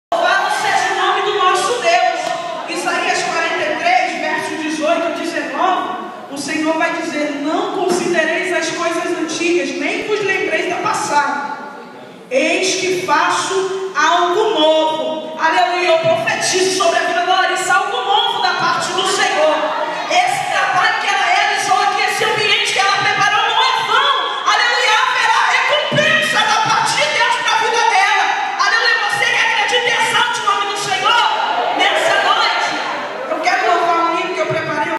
Culto de Gratidão da Larissa Mendes - Adressa Emanuelle
Culto de Gratidão da Larissa Mendes
| Sábado 02/Abril/2022.
Assembleia de Deus em Cafundá/ Congregação boiúna/RJ.
| Pastor Local: Italo Borges e Cláudia Borges.